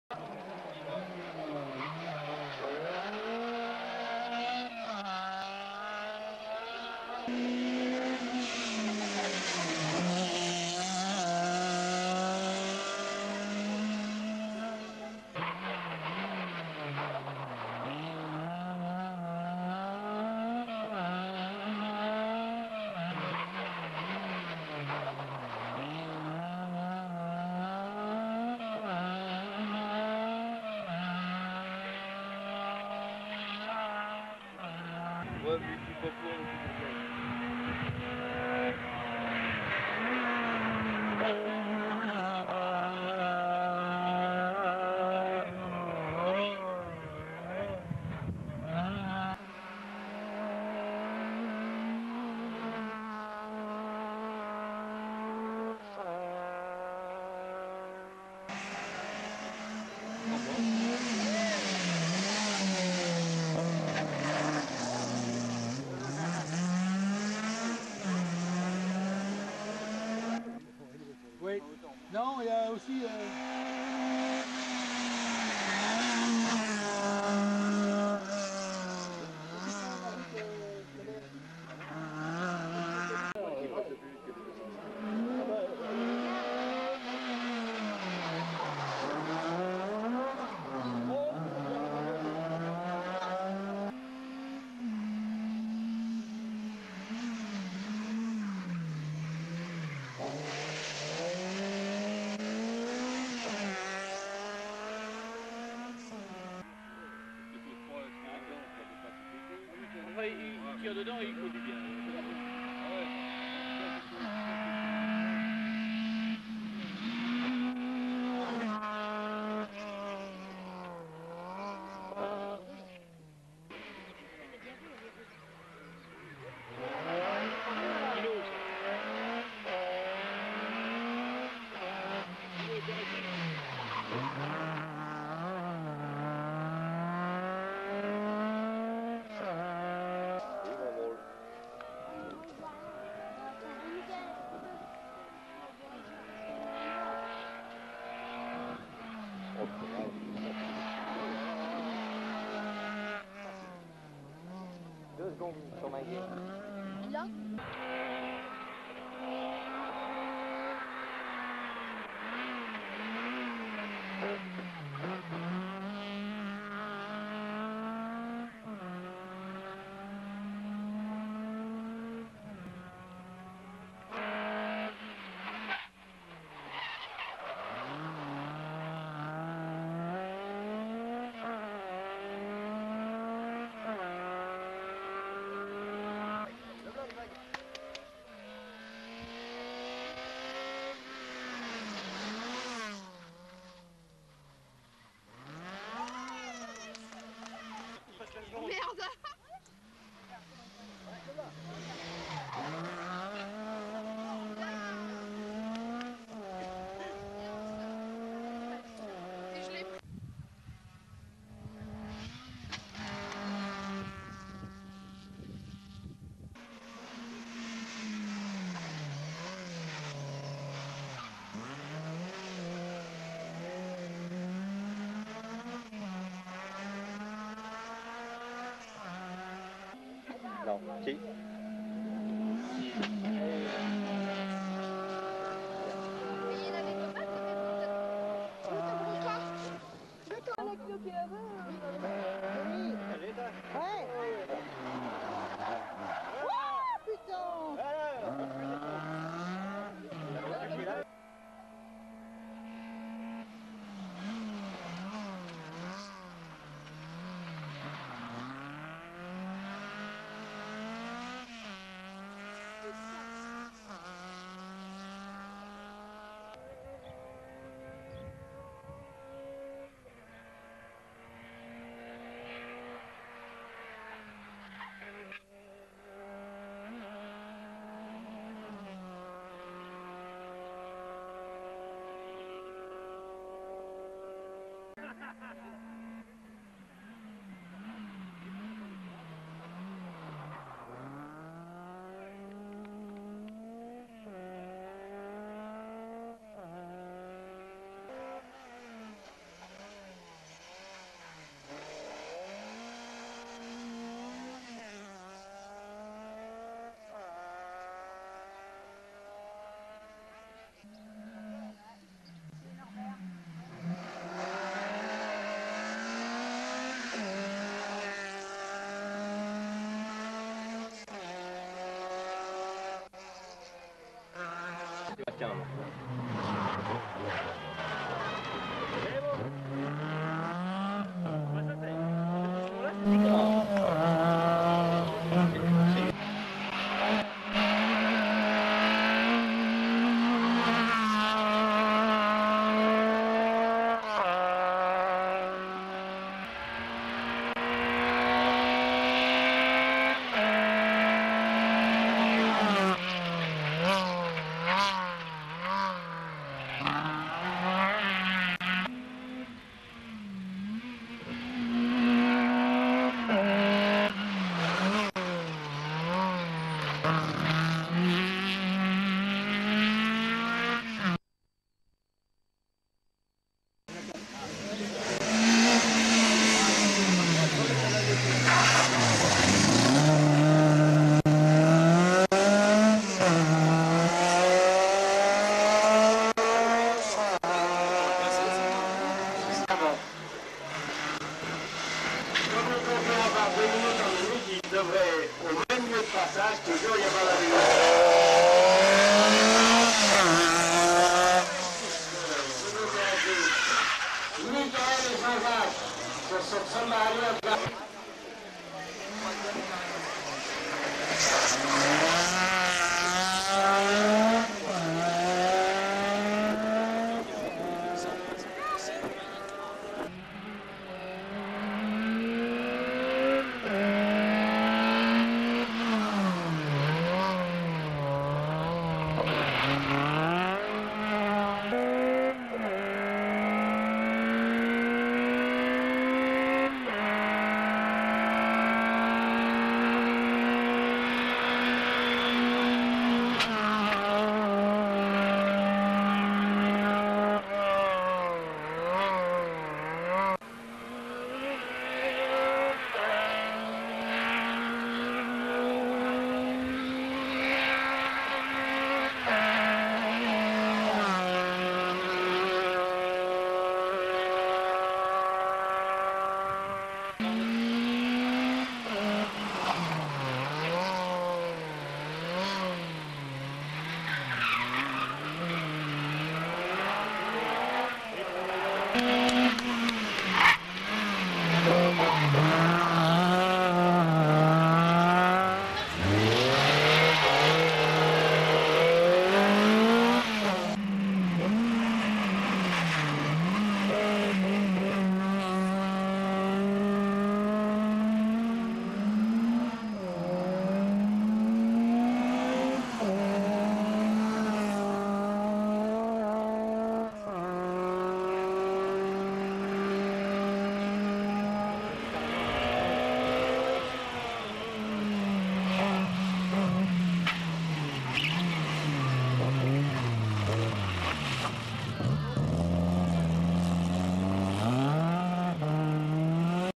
Best Of Guinot Mickael - Samba Rallye ex Gr.B- 1999 2003
Voici un Petit Best Of vidéo de la Samba Rallye Ex Gr.B sur les années 1999 a 2003
Équipage GUINOT Mickael / GUINOT J.Jack